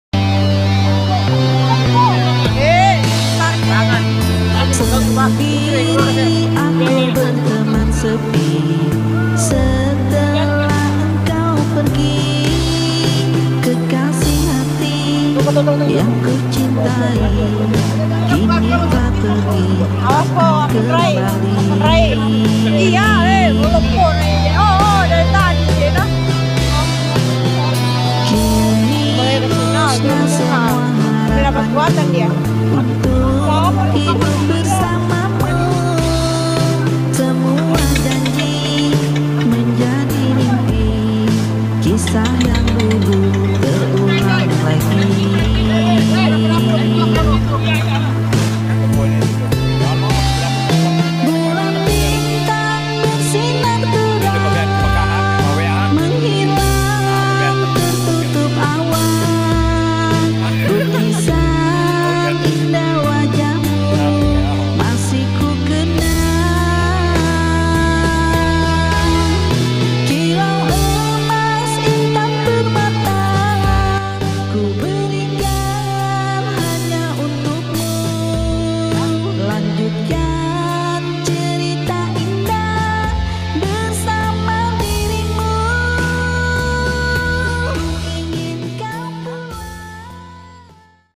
BUAYA BESAR PENUNGGU DANAU
buaya besar di tangkap warga di danau kaca